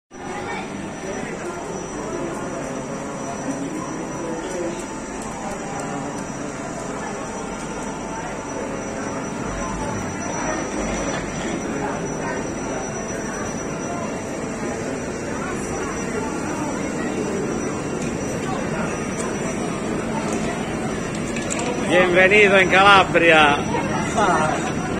L'arrivo di Ivan Caraballo in Calabria
Ivan sarà a Conflenti per due settimane durante le quali verrà a contatto con le tradizioni, gli usi e i costumi della sua terra di origine.